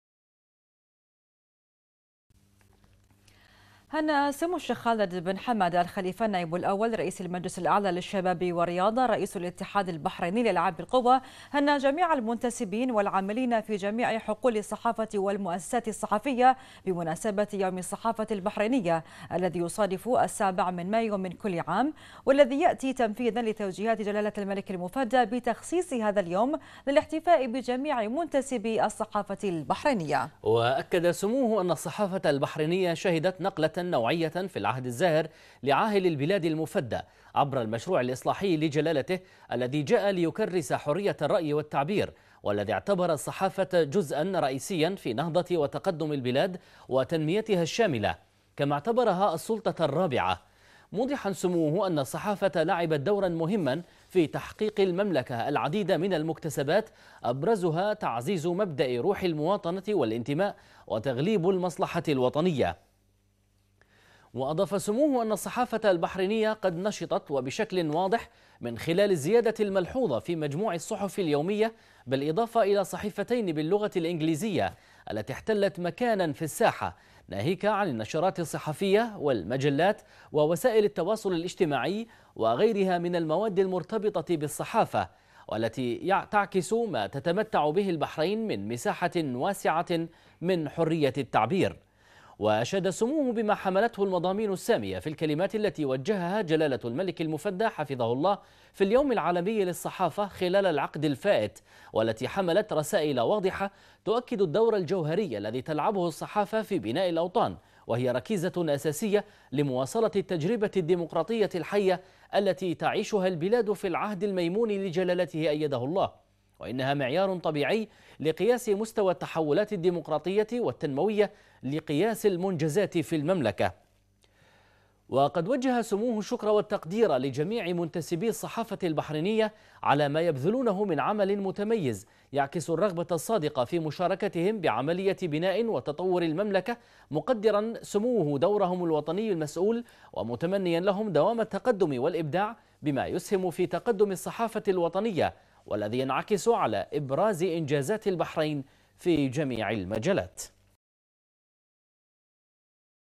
البحرين : سمو الشيخ خالد بن حمد يهنئ الأسرة الإعلامية بيوم الصحافة البحرينية
الوصف

 سمو الشيخ خالد بن حمد  يهنئ الأسرة الإعلامية بيوم الصحافة البحرينية